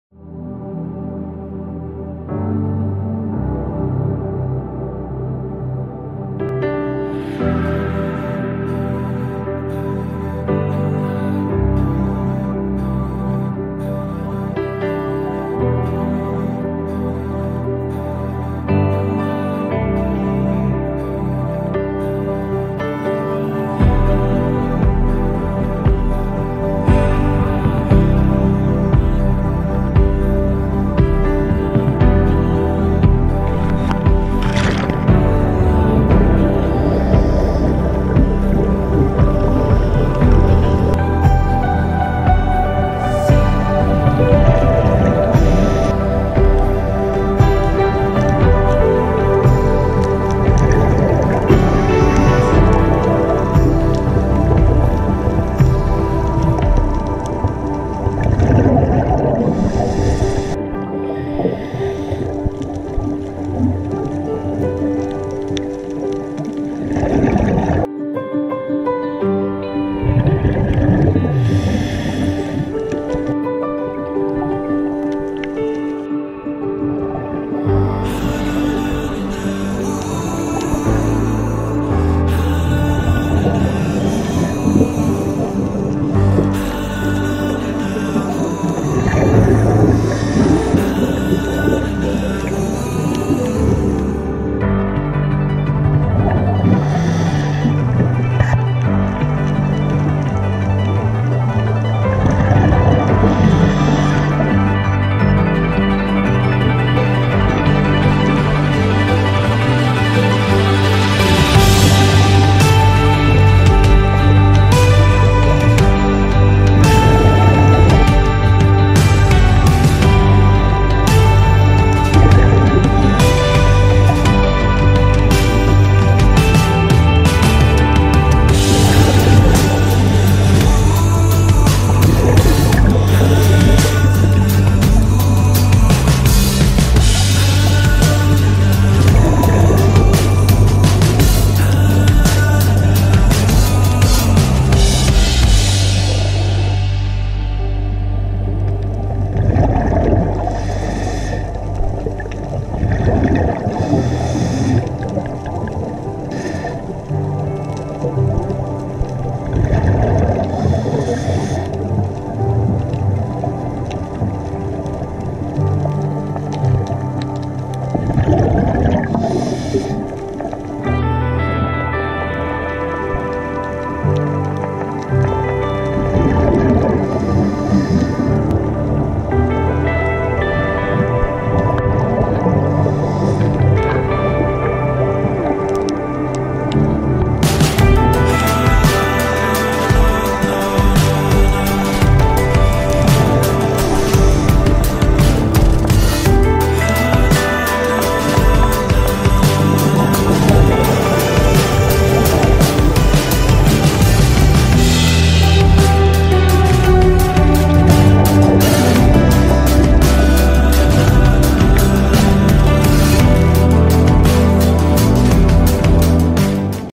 Trip To menjangan Island | Diving North Bali National Park
West Bali National Park is on the northwestern tip of Bali. The conservation area includes the mountainous Prapat Agung Peninsula and the surrounding coastal waters. Forests, mangroves and savanna protect rare animals like Bali starlings and bantengs, a species of wild cattle. Off the northeast shore of the peninsula, Menjangan Island is ringed by coral reefs and home to native barking deer and ancient Hindu temples